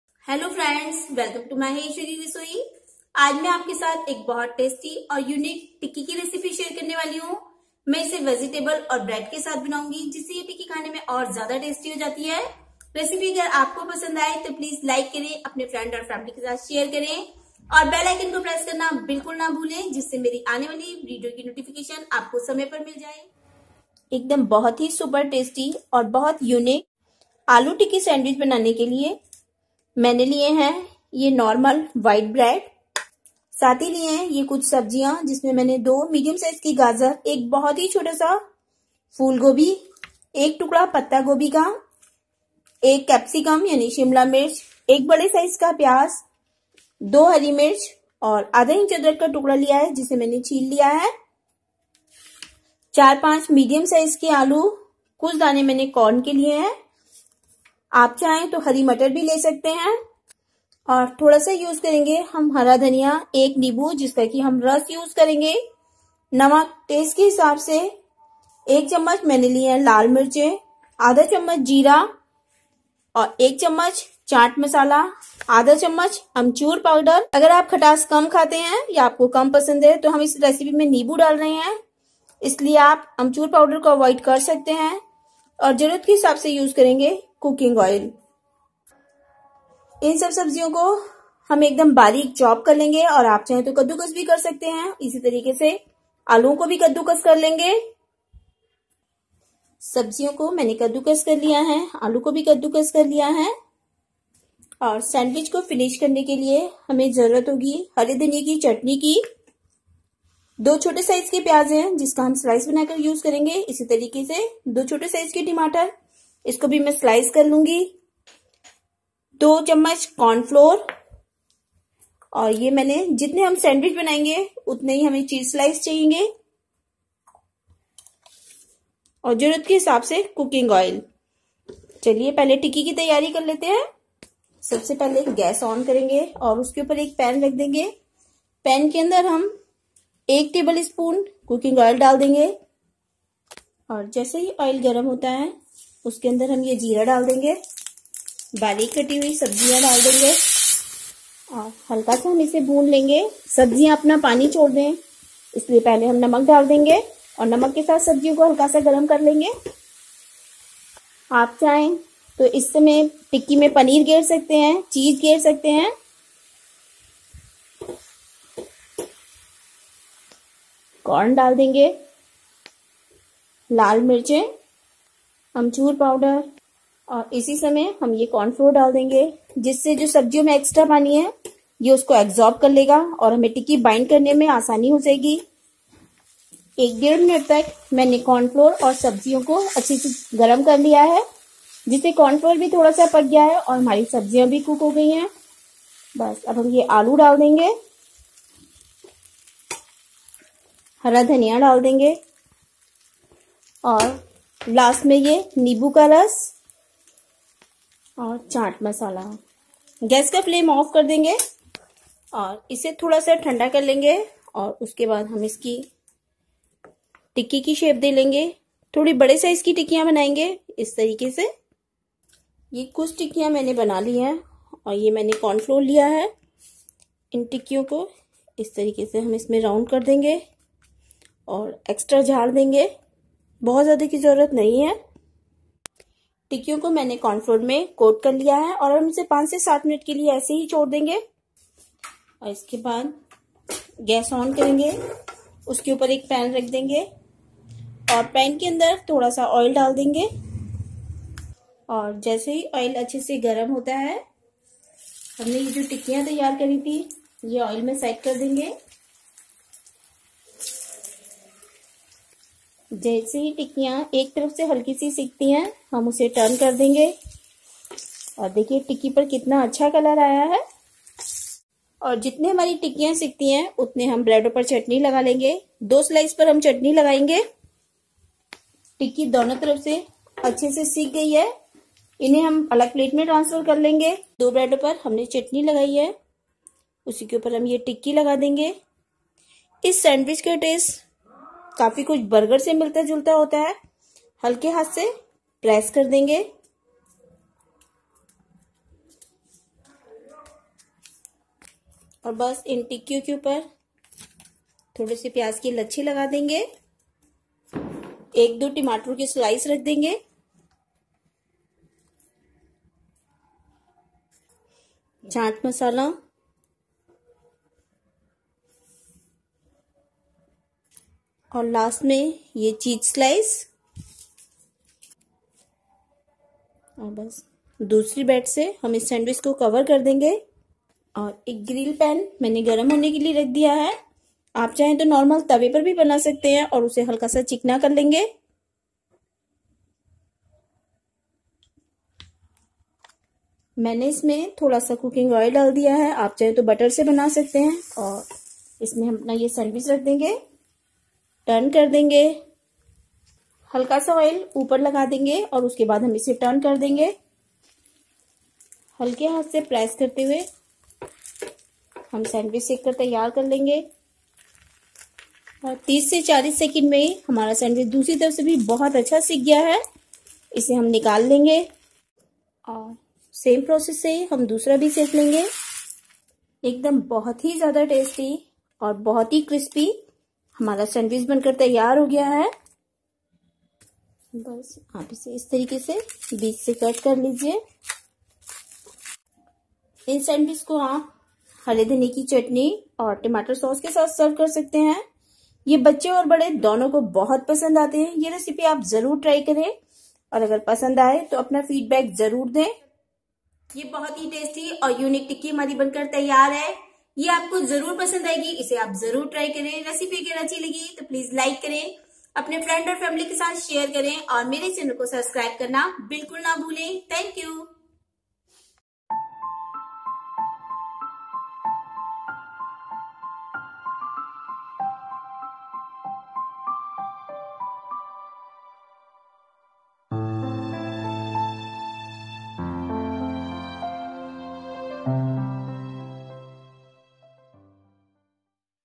एक बार सैंडविच बना लिया तो बाजार का बर्गर खाना भूल जाएंगे-Potato Tikki Sandwich- Aloo Tikki Sandwich
हेलो दोस्तों आज मैं आपके साथ एक सैंडविच की रेसिपी शेयर कर रही हूं मैं इसे आलू की टिक्की के साथ बनाऊंगी.. मुझे उम्मीद है आपको यह रेसिपी पसंद आएगी.